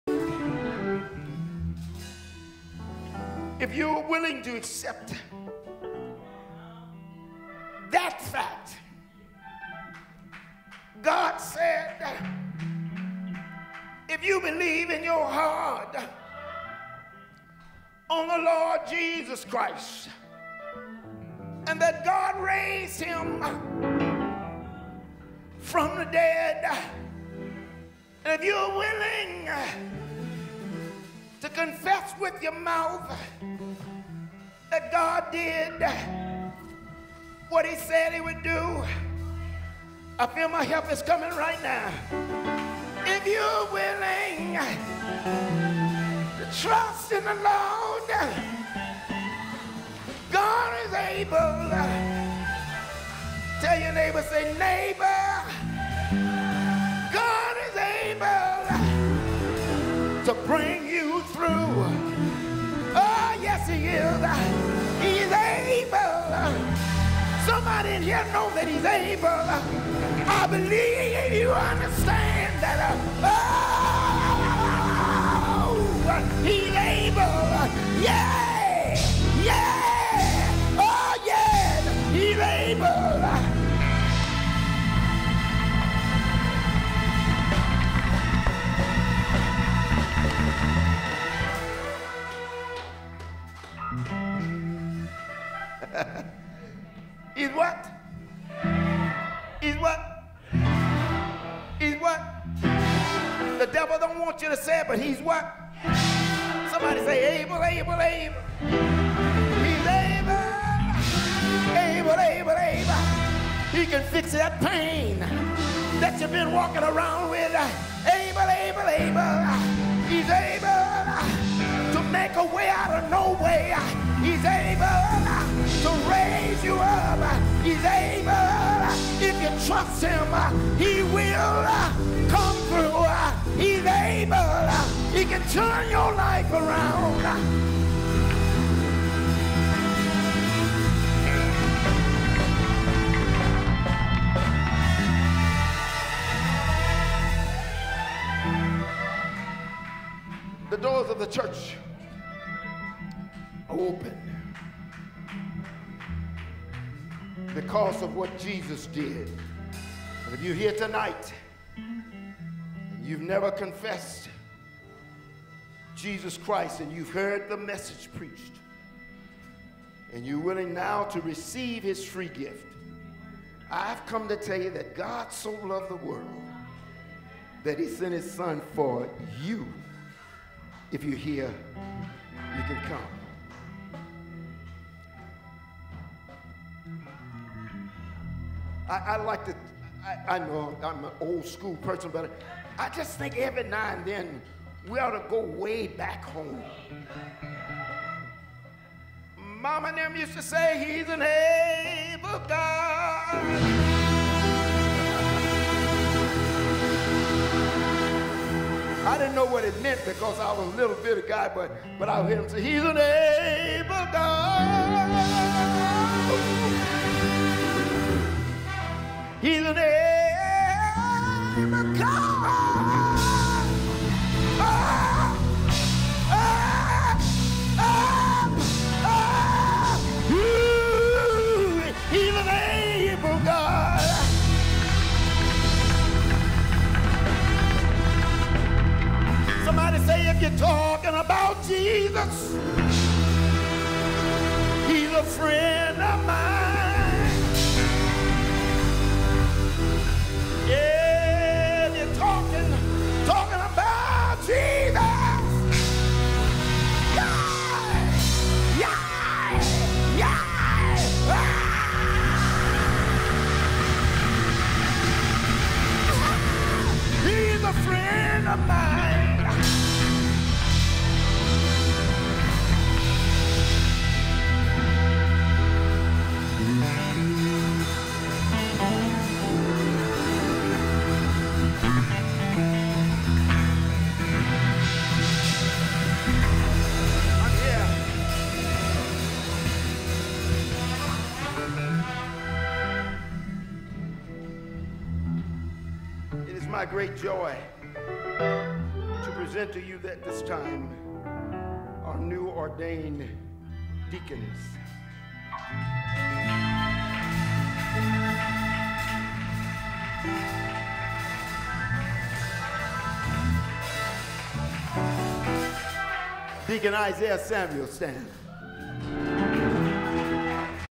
Abp Dr Sterling Lands II Closing at GCBC Austing Texas
Archbishop Dr. Sterling Lands II closing 03 February 2019 at the GCBC Deacons' Ordination service in Austin, Texas.